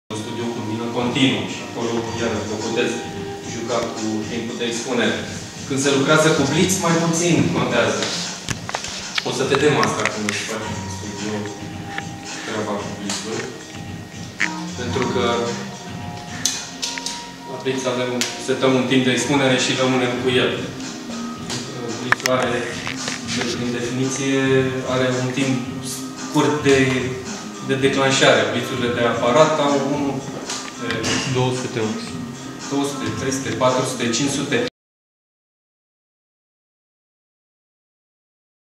Fotografie, Cosmin Gogu, DallesGO
Invatam despre fotografie pe traseul: perceptie, captare , prelucrare si interpretare
Genuri fotografice
Fotografia in familia artelor vizuale